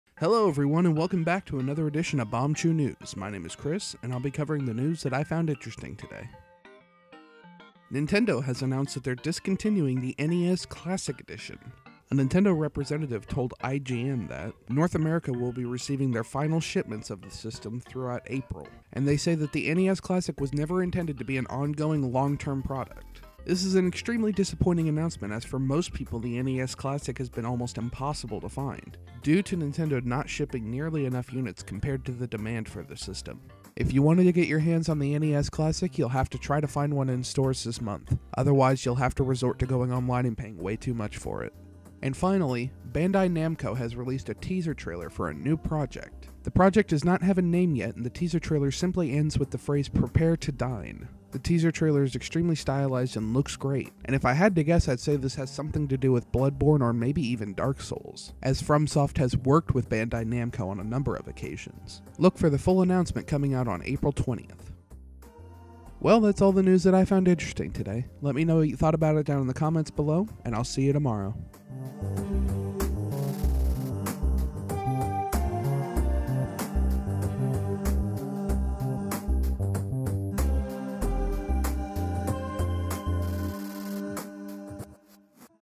R.I.P. NES Classic Edition | Bombchu News - April 11th, 2017
The news we found interesting today! Nintendo discontinues the NES Classic Edition, and Bandai Namco teases a new project with PrepareToDine and stylistic teaser trailer.

Loved the video and want to see even more gaming content?